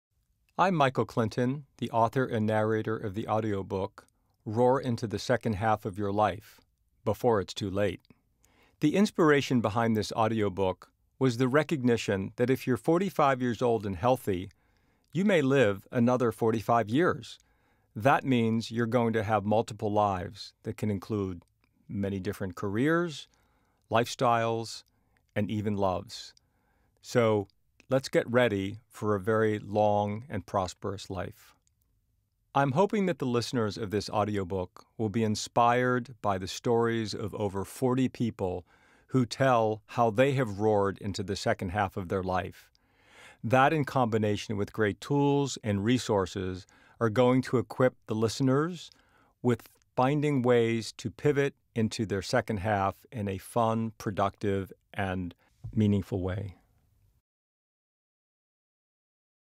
Michael Clinton talks about his audiobook ROAR
Watch Michael Clinton discuss his audiobook ROAR.